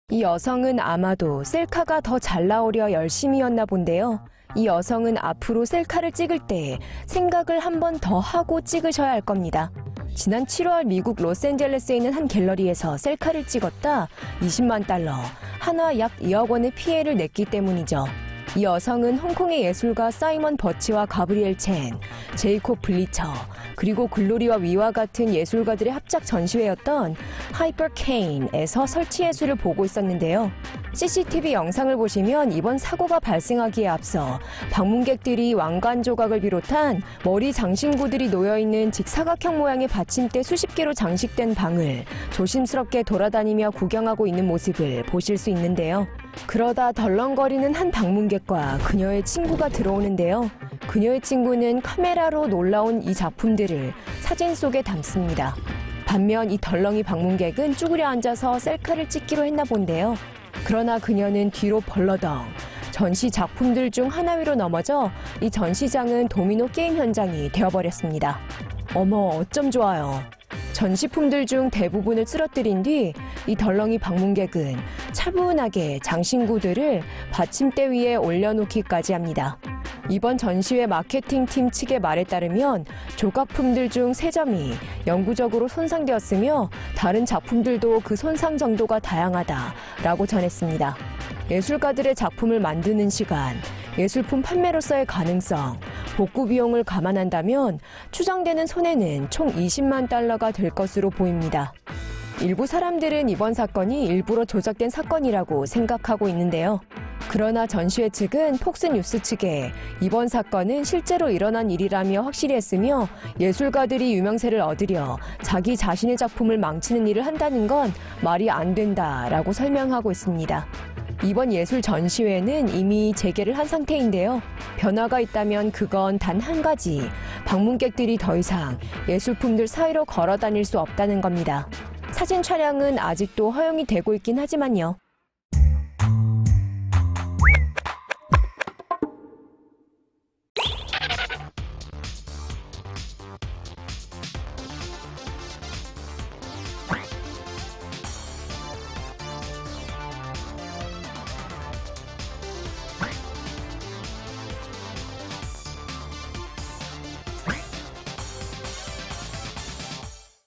한 여성, 예술품 전시회에서 셀카찍다, 예술품들 도미노처럼 다다다
캘리포니아, 로스앤젤레스 — 이 여성은 아마도 셀카가 더 잘나오려 열심히였었나 본데요...

이 여성은 앞으로 셀카를 찍을 때 생각은 한 번 더 하고 찍으셔야 할겁니다. 지난 7월 미국, 로스앤젤레스에 있는 한 갤러리에서 셀카를 찍었다, 20만달러, 한화 약 2억원의 피해를 냈기 때문입니다.

이 여성은 홍콩의 예술가 사이먼 버치와 가브리엘 챈, 제이콥 블리쳐 그리고 글로리아 위와 같은 예술가들의 합작전시회였던 ‘Hypercaine’에서 설치예술을 보고있었습니다.

 CCTV 영상을 보시면, 이번 사고가 발생하기에 앞서, 방문객들이 왕관 조각을 비롯한 머리 장신구들이 놓여져있는 직사각형 모양의 받침대 수십개로 장식된 방을 조심스럽게 돌아다니며 구경하고 있는 모습을 보실 수 있는데요,

그러다 덜렁거리는 방문객과 그녀의 친구가 들어옵니다. 그녀의 친구는 카메라로 놀라운 이 작품들을 사진에 담습니다.

반면, 이 덜렁이 방문객은 쭈그려 앉아서 셀카를 찍기로 했나본데요, 그러나 그녀는 뒤로 벌러덩 전시작품들 중 하나 위로 넘어져, 이 전시장은 도미노 게임 현장이 되어버렸습니다. 어머, 어쩜 좋아요...

전시품들 중 대부분을 쓰러트린 뒤, 이 덜렁이 방문객은 차분하게 장신구들을 받침대 위에 올려놓기까지 합니다.

이번 전시회 마케팅 팀 측의 말에 따르면, 조각품들 중 3점이 ‘영구적으로 손상되었으며, 다른 작품들도 그 손상정도가 다양하다.’라고 합니다. 예술가들의 작품을 만드는 시간, 예술품 판매로써의 가능성, 복구비용을 감안한다면, 추정되는 손해는 총 20만달러가 될 것으로 보입니다.

일부 사람들은 이번 사건이 일부러 조작된 사건이라고 생각하고 있습니다. 그러나 전시회 측은 폭스뉴스 측과 이번 사건은 실제로 일어난 일이라고 확실히 했으며, 예술가들이 유명세를 얻으려 자기자신의 작품을 망치는 일을 한다는 건 말이 안된다로 설명하고 있습니다.

이번 예술 전시회는 이미 재개를 한 상태입니다. 변화가 있다면 그건 단 한가지, 방문객들이 더이상 예술품들 사이로 걸어다닐 수 없다는 것입니다. 사진촬영은 아직도 허용이 되고있긴 하지만요.